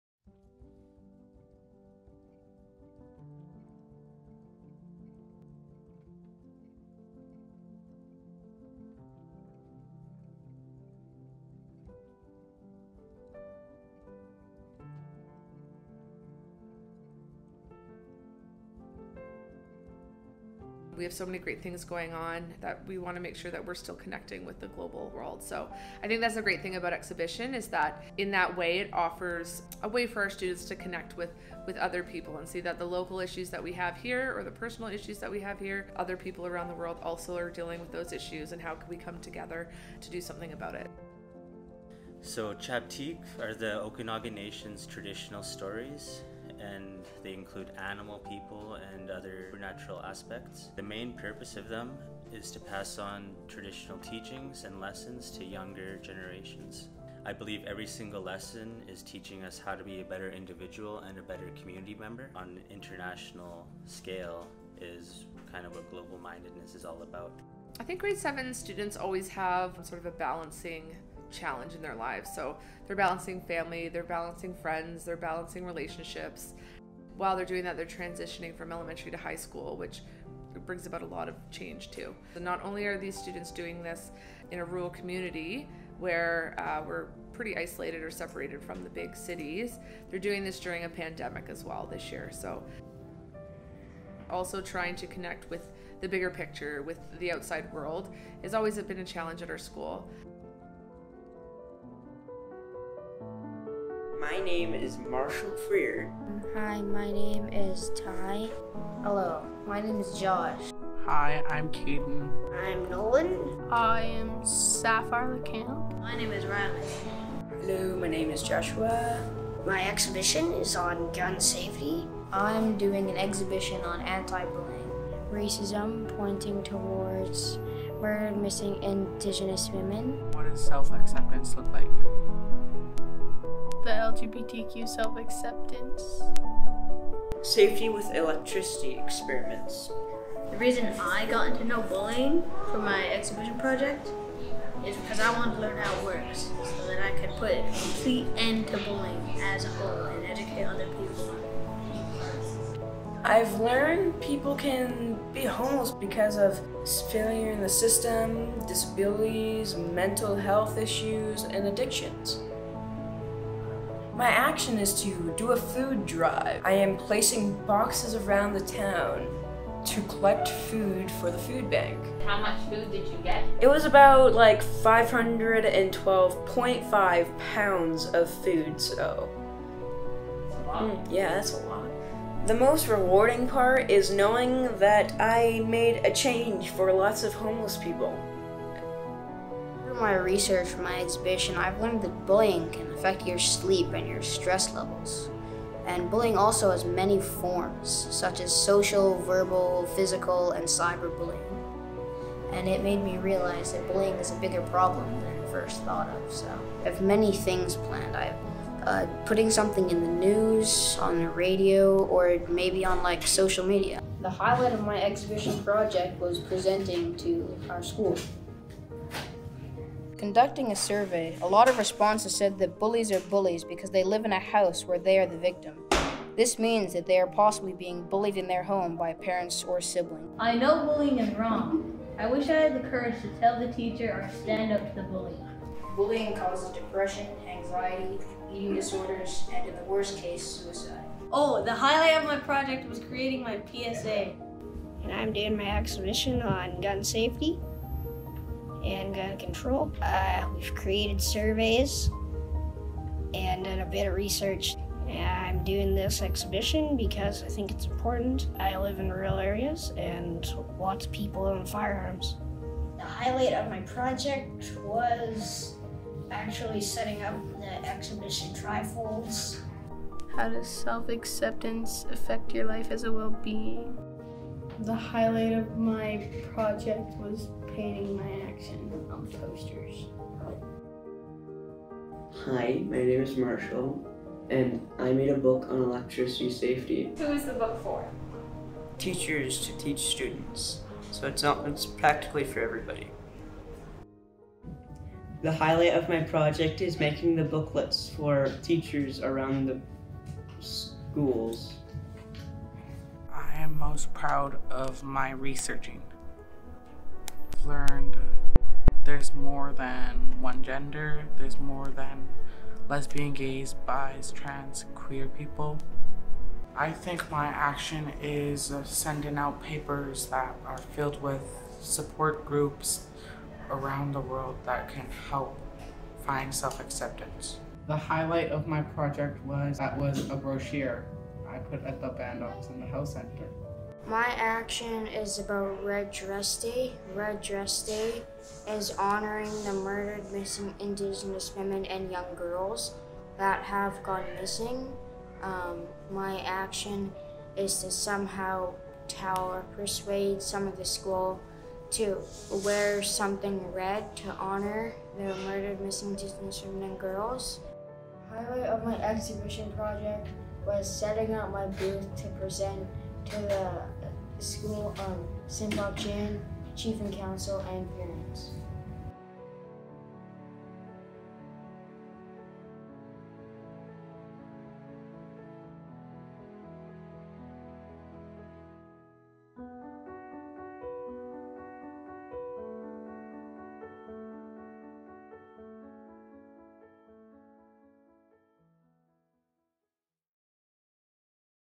Senpaq’cin School Exhibition Video
In 2021, grade 7 students at Senpaq’cin School created independent projects featuring social issues that are important to them. Each student saw their project through from conception to presentation, including action items for the betterment of the community, and recorded footage along the way. We helped put it together into this short documentary, which features their moving work on bullying, murdered and missing Indigenous women, self-acceptance, electricity and gun safety, and homelessness.